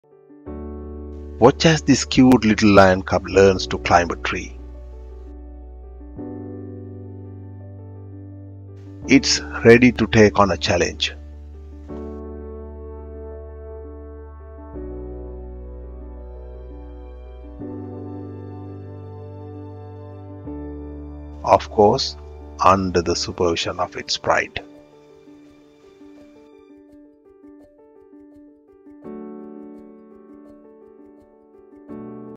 Training Under Supervision
Watch as this cute little lion cub learns to climb a tree. Its ready to take on a challenge. Of course under the supervision of its pride.